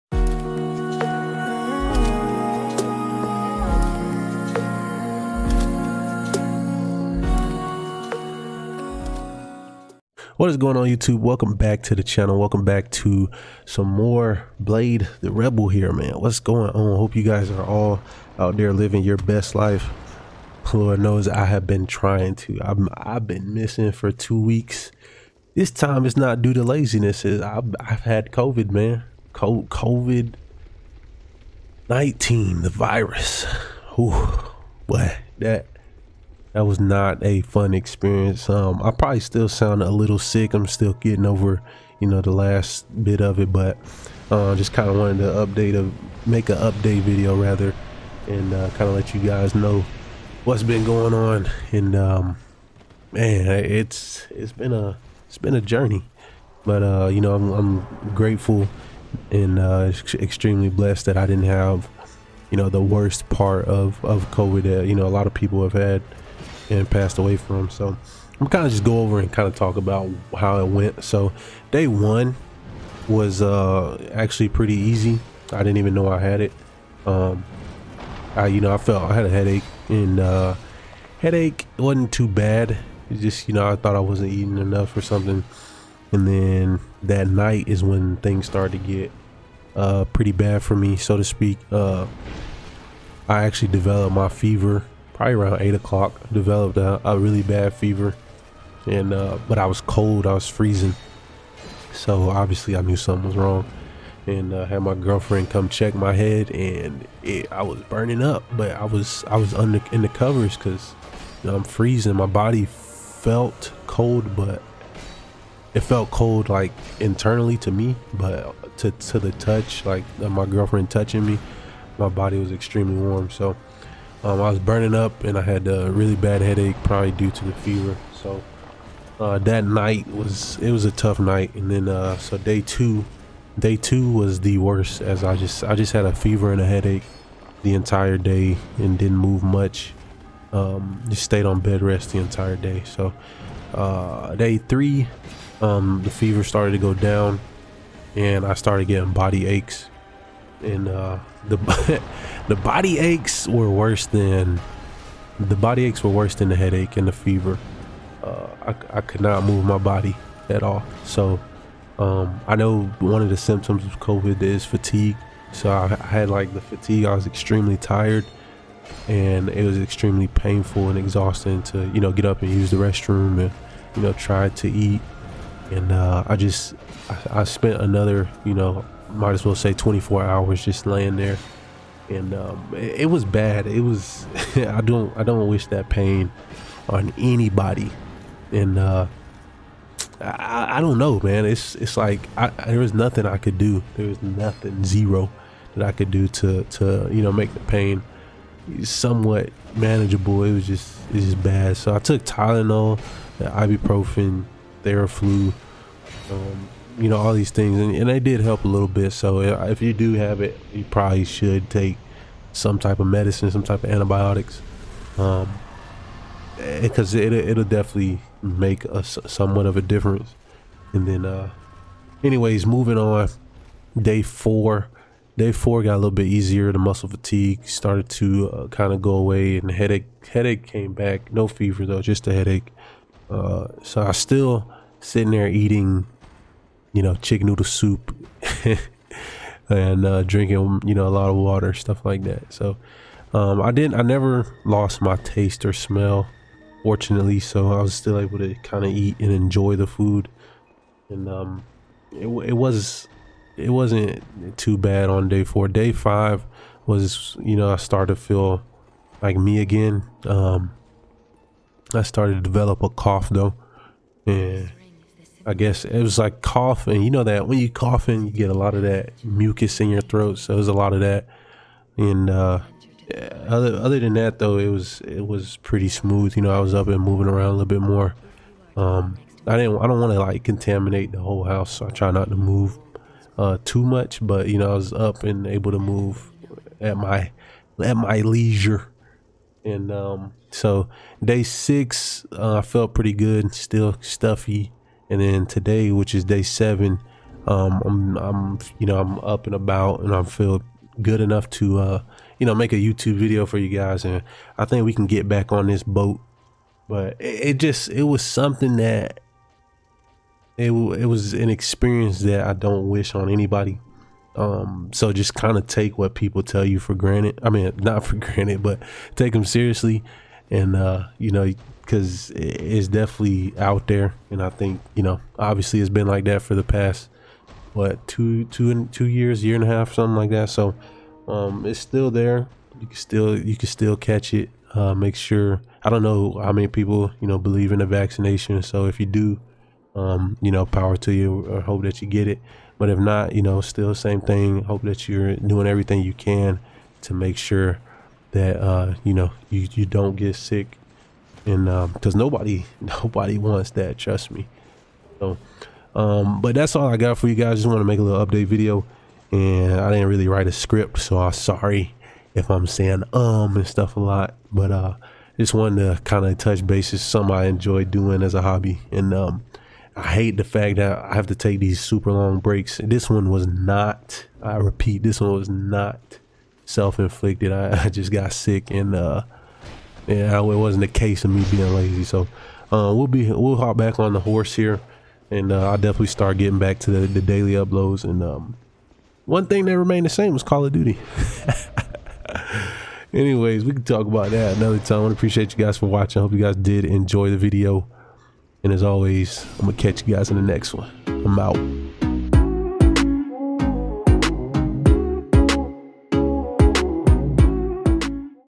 COVID SUCKS...
What's up YouTube, welcome to the channel. I'm finally back after having COVID. It sucks as bad as they say. Anyway, I hope you guys enjoy.

Gimme a FOLLOW! Please lol ❤️ 🖤
Updates on the channel are announced here...